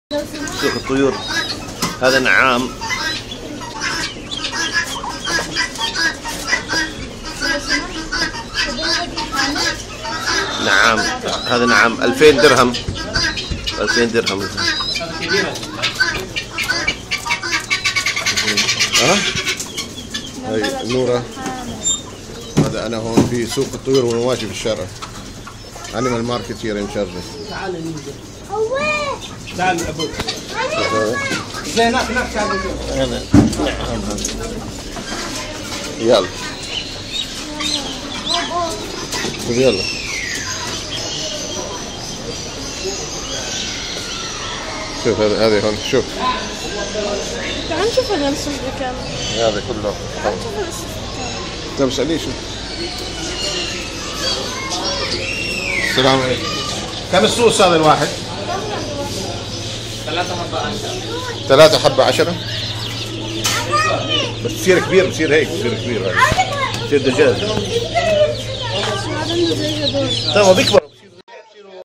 Birds at the Birds and Animals Market, Sharjah, UAE. 20.06.2012
Birds at the Birds and Animals Market, Sharjah, United Arab Emirates. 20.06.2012.

Video by: Dr. Sc. Norman Ali Bassam Khalaf.
Visiting the Market with my daughter Nora Norman Ali Khalaf.

With Samsung Galaxy Tab 7.7 Video Camera.